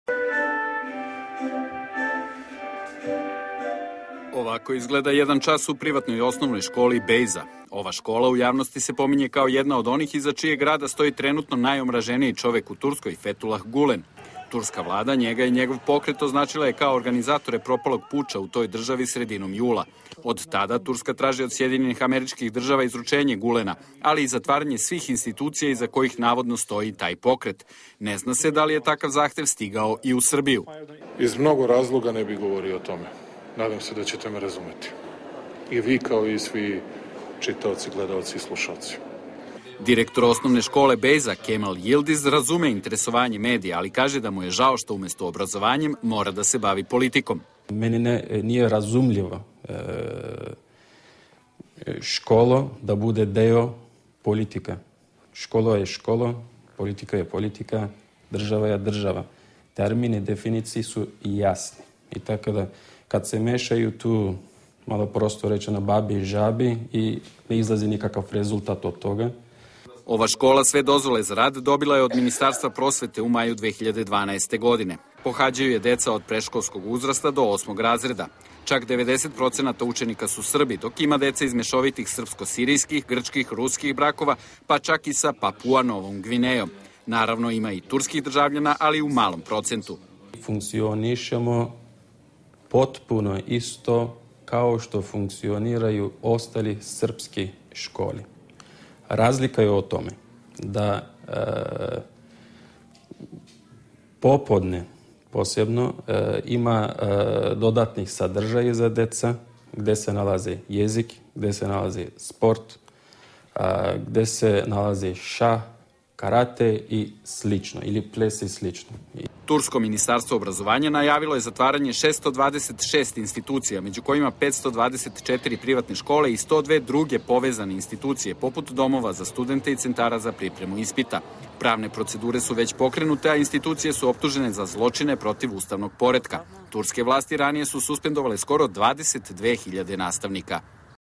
BG: Kako izgledaju časovi u školi turskog neprijatelja broj 1
Iako je ministar prosvete Mladen Šarčević rekao da je od turskog ambasadora dobio zahtev da se u Srbiji zatvore škole iza kojih navodno stoji Fetulah Gulen, premijer nije mogao da potvrdi da je takav zahtev stigao iz Turske. Naša ekipa posetila je jednu takvu privatnu školu u Beogradu.